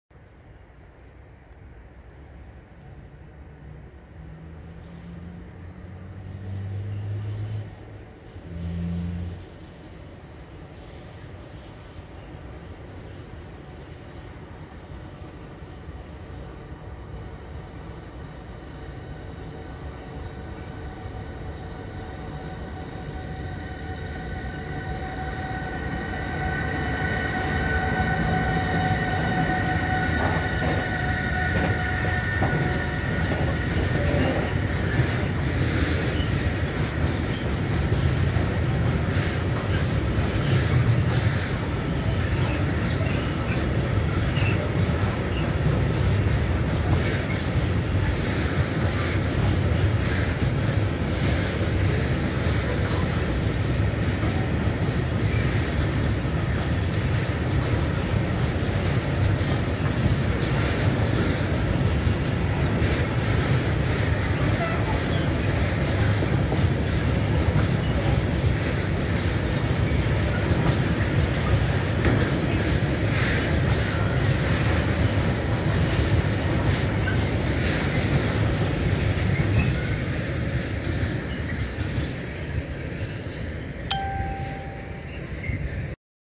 E6ACT-008 z cysternami
E6ACT-008 Lotos z cysternami, mija stację Poznań Górczyn.